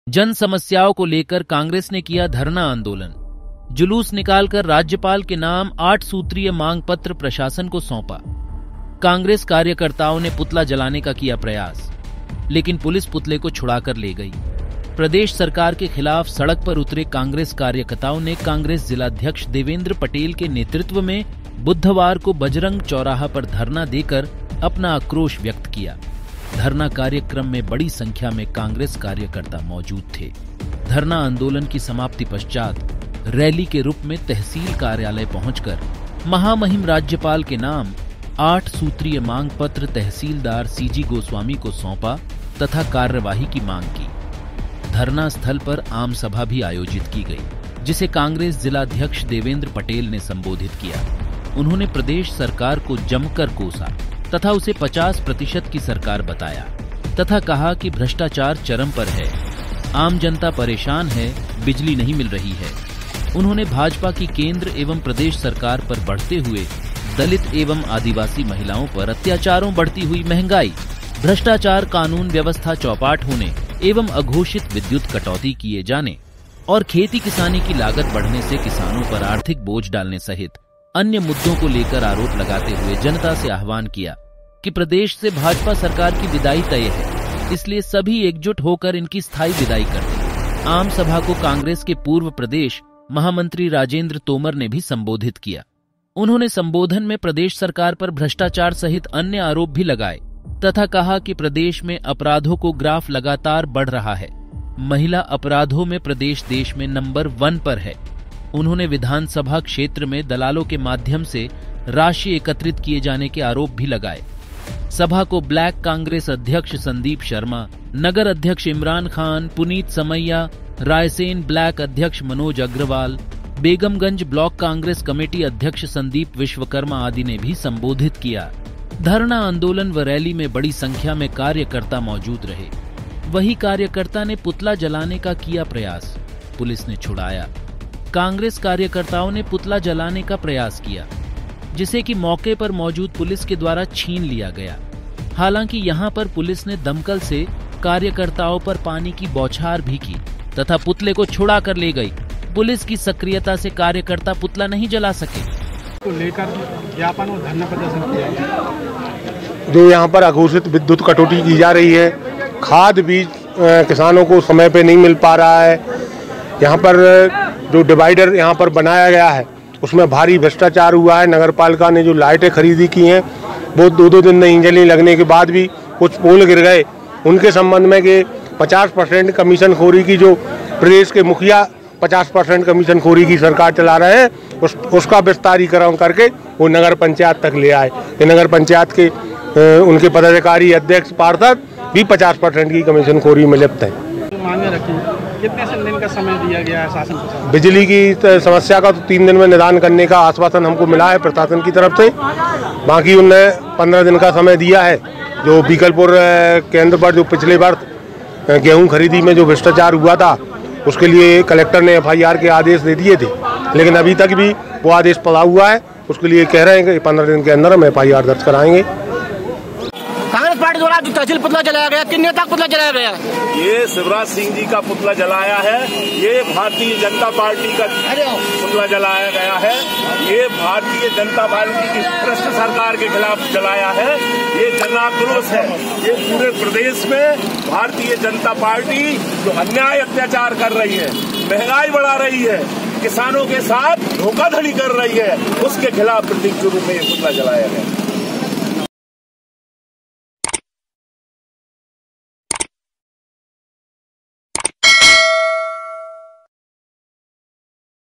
सिलवानी में Congress का धरना आंदोलन | CM शिवराज का पुतला जलाने की कोशिश | Police ने छुड़ाया
जन समस्याओ को लेकर कांग्रेस ने किया धरना आंदोलन।
जुलूस निकाल कर राज्यपाल के नाम आठ सूत्रीय मांग पत्र प्रशासन  को सौंपा।

कांग्रेस कार्यकर्ताओ ने पुतला जलाने का किया प्रयास। लेकिन पुलिस पुतले को छुड़ा कर ले गई।

 प्रदेश  सरकार के खिलाफ सड़क पर उतरे कांग्रेस कार्यकताओ ने कांग्रेस जिलाध्यक्ष देवेंद्र पटेल के नेतृत्व में बुधवार को बजरंग चौराहा पर धरना देकर अपना आक्रोश  व्यक्त किया। धरना कार्यक्रम में बड़ी संख्या में कांग्रेस कार्यकर्ता मौजूद थे। धरना आंदोलन की समाप्ति पश्चात  रैली के रुप में तहसील कार्यालय पहुंच कर महामहिम राज्यपाल के नाम आठ सूत्रीय मांग पत्र तहसीलदार सीजी गोस्वामी को सौंपा तथा कार्रवाही की मांग की। धरना स्थल पर आमसभा भी आयोजित की गई। जिसे कांग्रेस जिलाध्यक्ष देवेंद्र पटेल ने संबोधित किया। उन्होने प्रदेश  सरकार को जमकर कोसा। तथा उसे पचास
प्रतिषत की सरकार बताया। तथा कहा कि भ्रष्टाचार चरम पर है। आम जनता परेशान  है, बिजली नही मिल रही है। उन्होने भाजपा की केंद्र एवं प्रदेश सरकार पर बढ़ते हुए दलित एवं आदिवासी महिलाओं पर अत्याचारों बढ़ती हुई महंगाई भ्रष्टाचार कानून व्यवस्था चौपाट होने एवं अघोषित विद्युत कटौती किए जाने और खेती किसानी की लागत बढ़ने से किसानों पर आर्थिक बोझ डालने
सहित अन्य मुद्दों को लेकर आरोप लगाते हुए जनता से आह्वान किया कि प्रदेश से भाजपा सरकार की विदाई तय है इसलिए सभी एकजुट होकर इनकी स्थाई बिदाई कर दे। आम सभा को कांग्रेस के पूर्व प्रदेष महामंत्री राजेंद्र तोमर ने भी संबोधित किया। उन्होने संबोधन में प्रदेश सरकार पर भ्रष्टाचार सहित अन्य आरोप भी लगाए तथा कहा कि प्रदेश  मेें अपराधो को ग्राफ लगातार बढ़ रहा है। महिला अपराधो में प्रदेश  देश में नंबर वन पर है। उन्होने विधानसभा क्षेत्र में दलालो के माध्यम से राषि एकत्रित किए जाने के आरोप भी लगाए। सभा को ब्लाक कांग्रेस अध्यक्ष संदीप शर्मा, नगर अध्यक्ष इमरान खान, पुनीत समैया, रायसेन ब्लाक अध्यक्ष मनोज अग्रवाल, बेगमगंज ब्लॉक कांग्रेस कमेटी अध्यक्ष संदीप विश्वकर्मा आदि ने भी संबोधित किया। धरना आंदोलन व रैली में बड़ी संख्या में कार्यकर्ता मौजूद रहें। वही कार्यकर्ता ने पुतला जलाने का किया प्रयास , पुलिस ने छुड़ाया, कांग्रेस  कार्यकर्ताओ ने पुतला जलाने का प्रयास किया। जिसे कि मौके पर मौजूद पुलिस के द्वारा छीन लिया गया। हालांकि यहां पर पुलिस ने दमकल से कार्यकर्ताओ पर पानी की बौछार भी की।  तथा पुतले को छुड़ा कर ले गई। पुलिस की सक्रियता से कार्यकर्ता पुतला नही जला सके।

KESRINEWS
political party
Mp police
cm shivraj singh chouhan
Mp news
Silwani raisen
raisen news

राहुल गांधी

congress party video

rahul gandhi